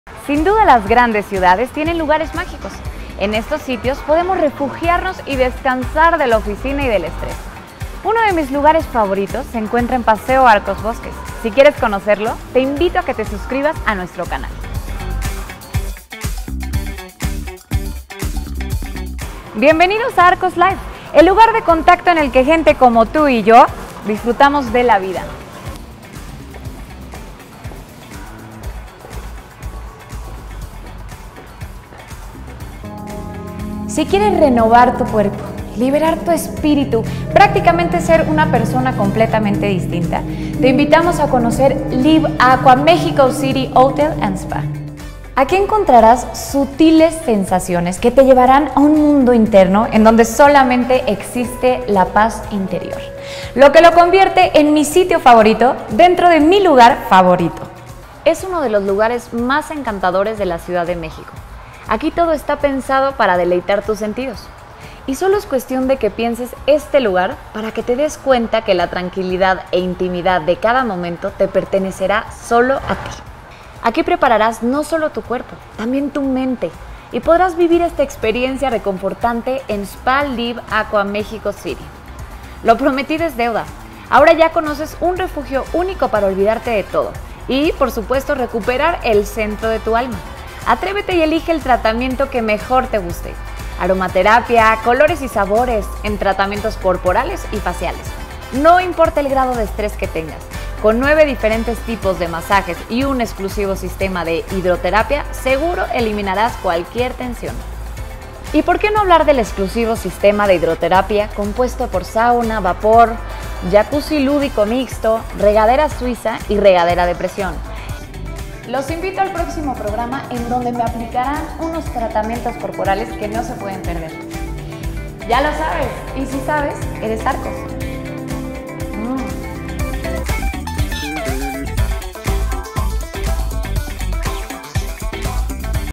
Arcos Life/ Vive Una Experiencia Única / Spa Feel Aqua
Consiente tu cuerpo y pasa días memorables, en Feel Aqua Spa, claro en el hotel Live Aqua en Paseo Arcos Bosques, que esperas haz tu reservación ya, y suscríbete a tu canal Arcos Life.